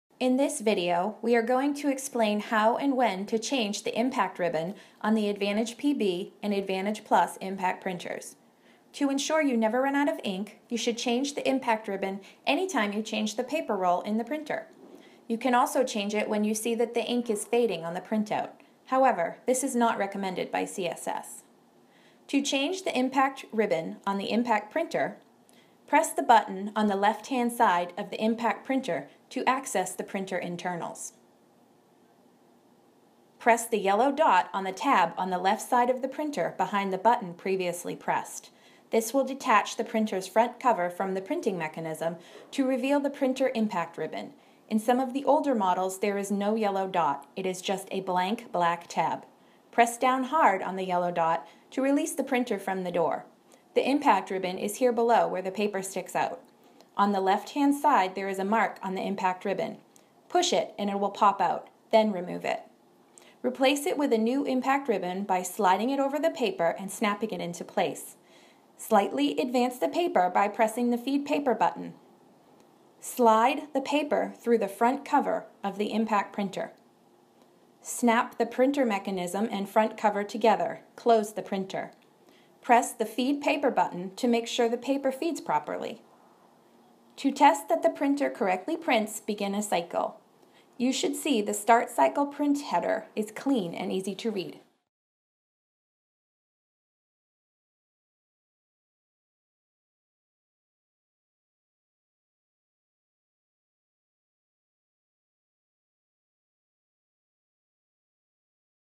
How to Change Your Autoclave's Printer Ribbon - Advantage Series
Learn how and when to change the impact ribbon on your ADV-PB/PLUS Control Systems.